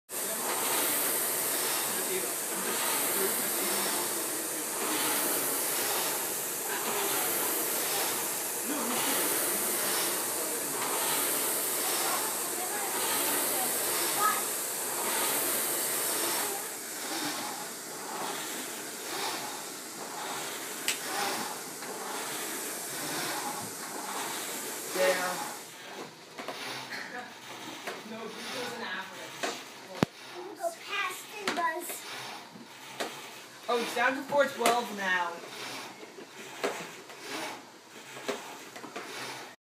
The New Toy
Cannot believe we just bought this huge thing to go in the new basement.  Hope it gets used A LOT.